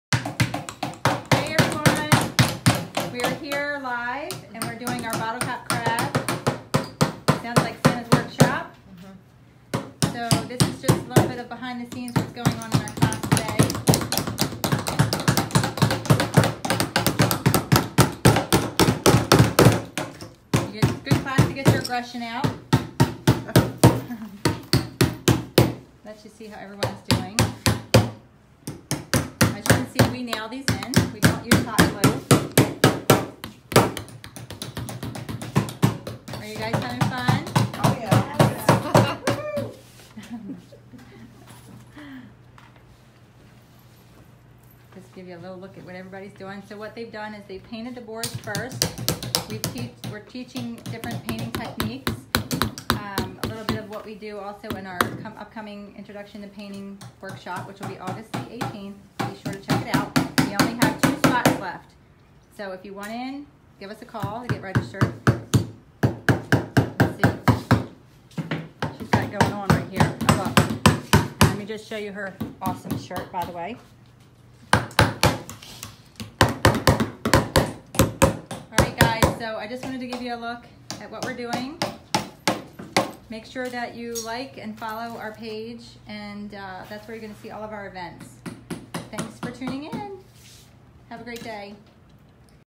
Bottle Cap Crab Workshop
We had a ball at our Bottle cap crab workshop yesterday. Everyone did a fantastic job!!
Find more info about upcoming workshops on our Facebook. The Rustic Peddler.